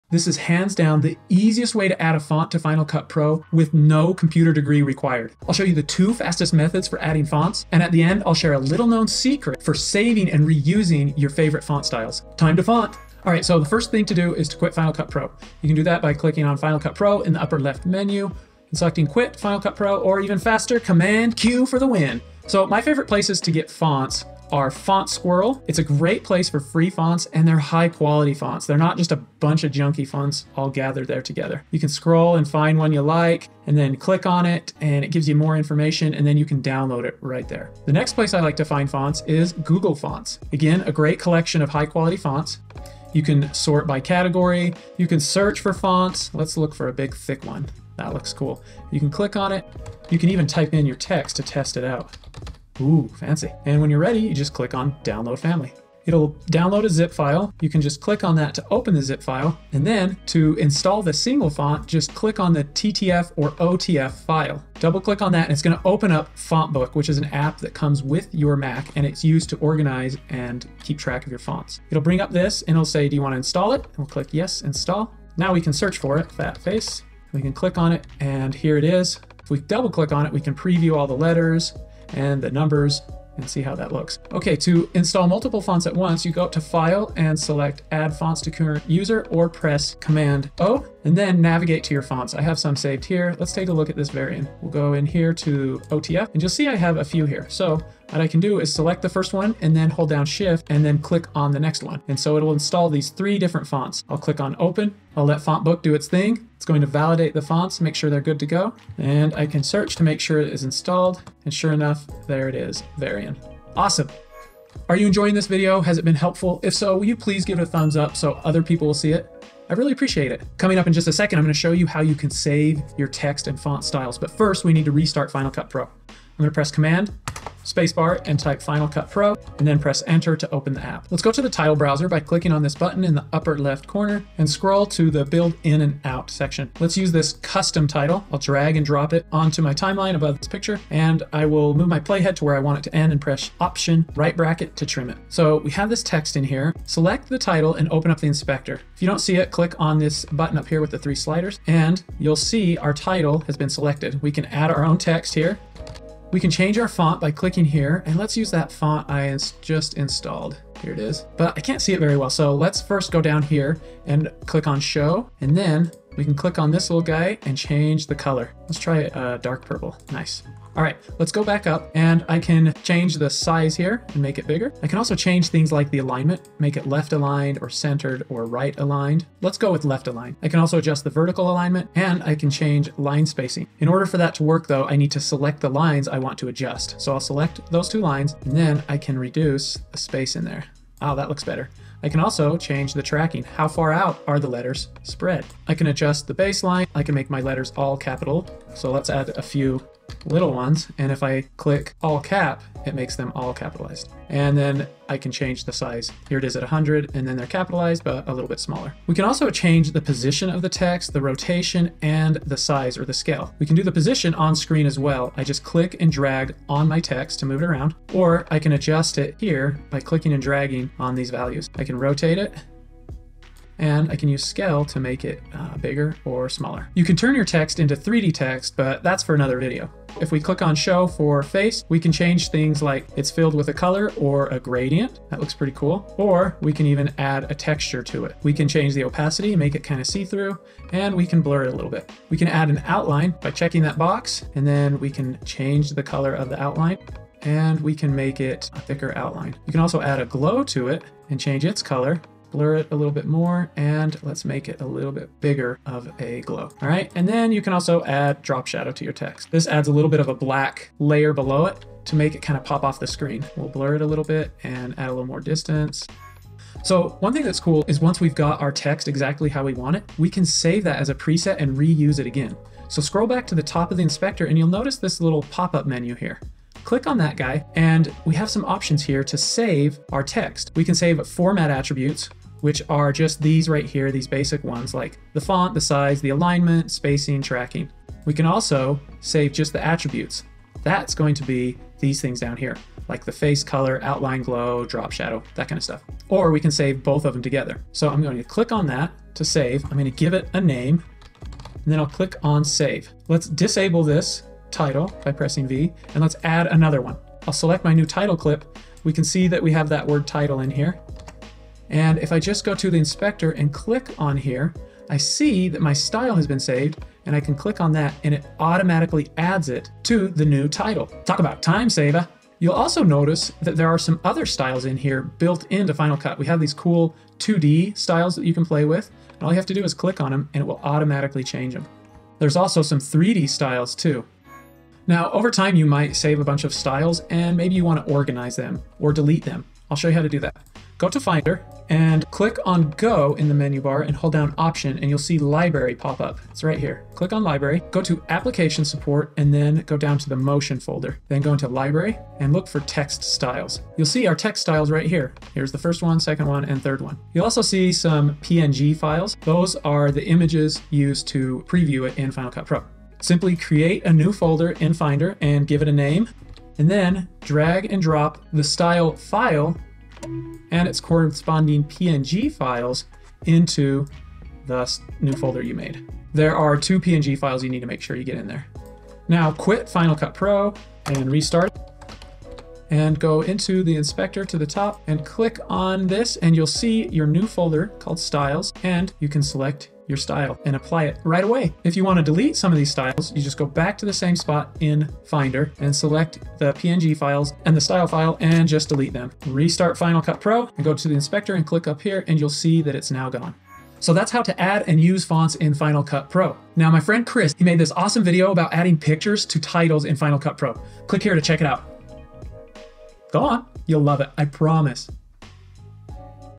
How to Add a Font to Final Cut Pro
This is hands down the easiest way to add fonts to Final Cut Pro with no computer degree required! I’ll show you the 2 fastest methods for adding fonts and at the end I’ll share a little known secret for saving and reusing your favorite font styles. Time to font!

00:00 - Intro
00:12 - Quit FCP
00:23 - Find fonts
01:00 - Font Book
02:04 - Restart FCP
02:15 - Add a title
02:25 - Edit Text
04:25 - Text Styles and Attributes
06:43 - Outro